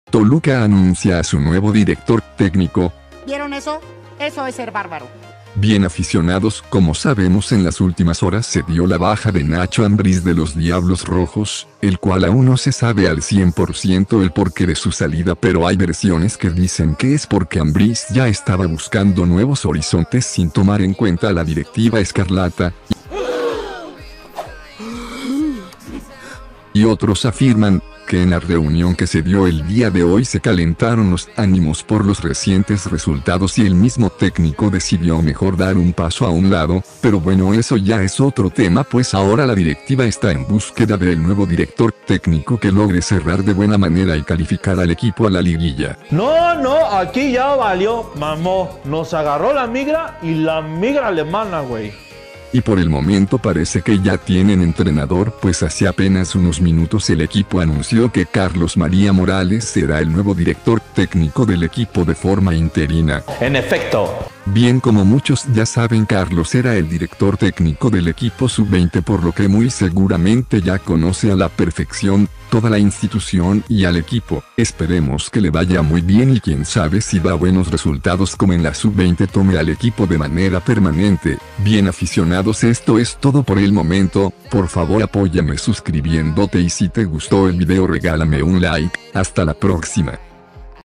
El TOLUCA ya tiene NUEVO DIRECTOR TÉCNICO
Hola saludos

Los diablos rojos de Toluca ya tienen nuevo director técnico para sustituir a Nacho Ambriz